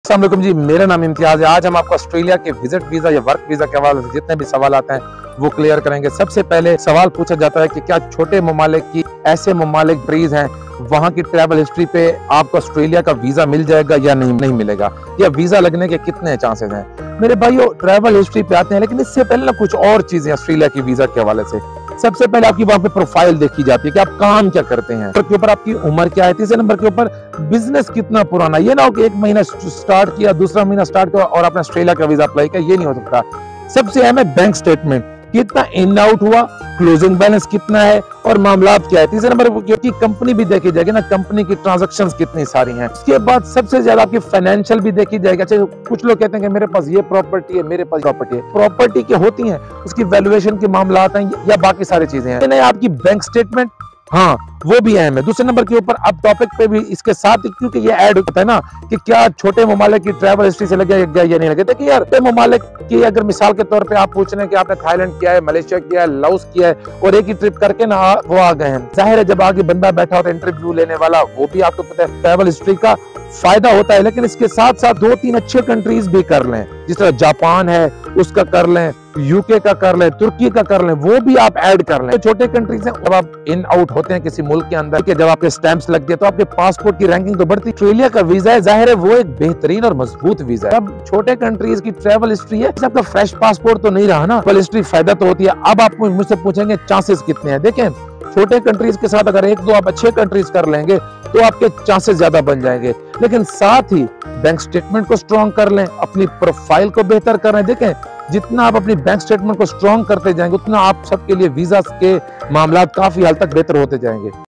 How to get australia work visa visit visa on small countries travel history from Pakistan
pakistan to australia visa kay hawalay say bataya hay travel history aur baqi kaya aisi chezen karain kah visa lag jaye